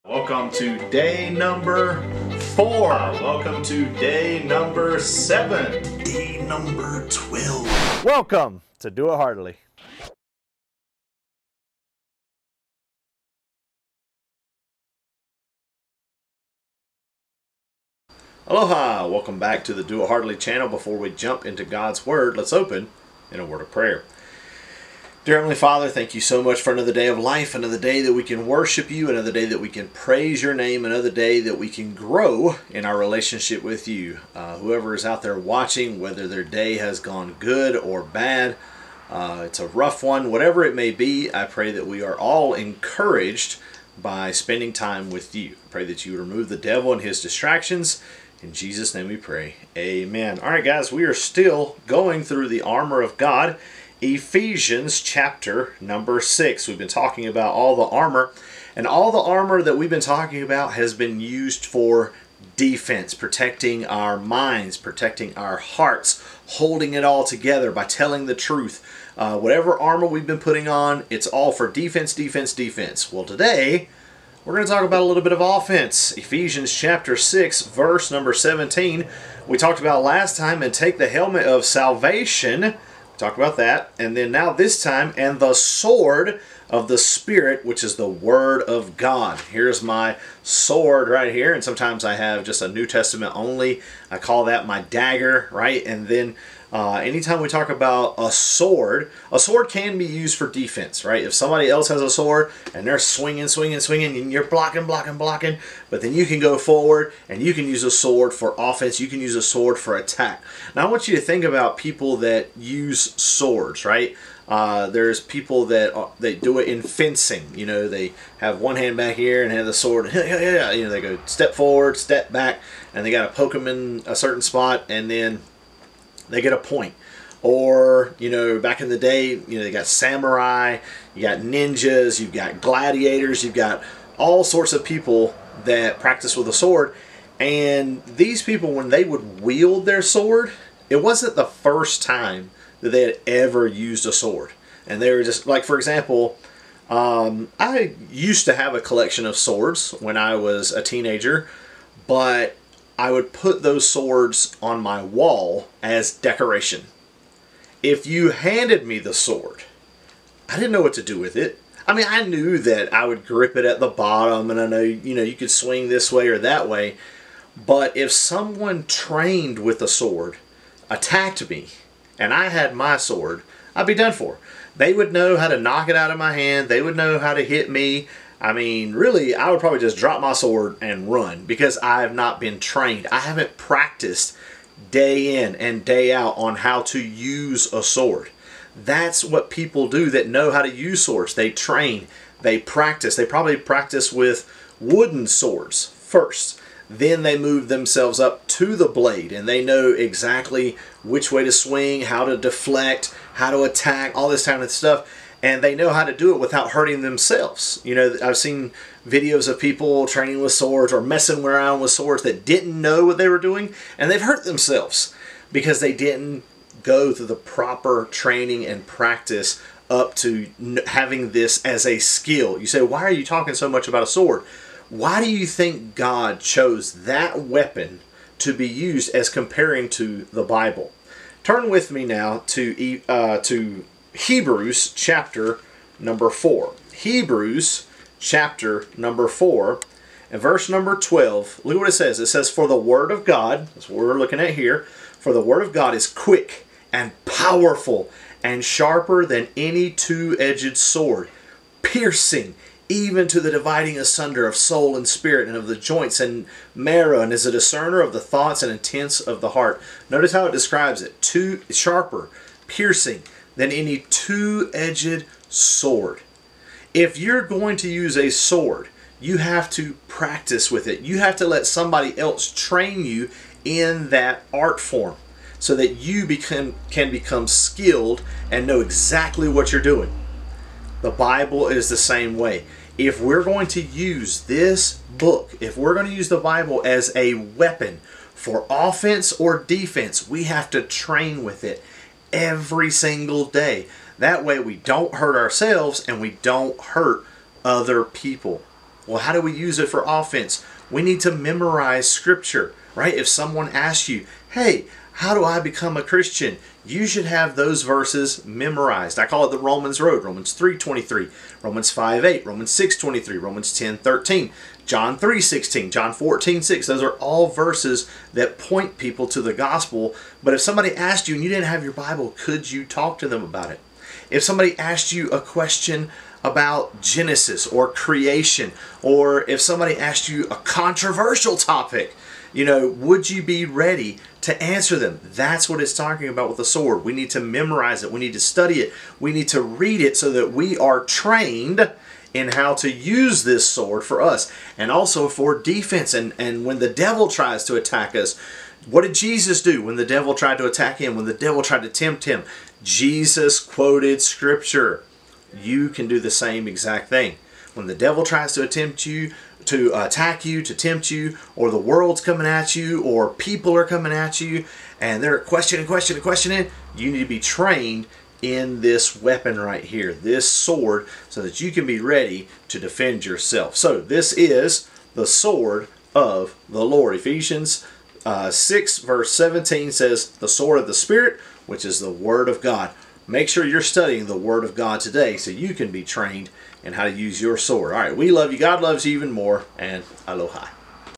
Day 336 Sword of the Spirit
1. How do we wear the belt?
2. How do we wear the breastplate?
3. How do we wear the shoes?
4. How do we use the shield?
5. How do we wear the helmet?
6. What do we need to do with the Bible that people do when they want to use a sword?
7. Am I good with a sword?
8. How does Hebrews describe the Bible as a sword?
9. Did you watch the whole video?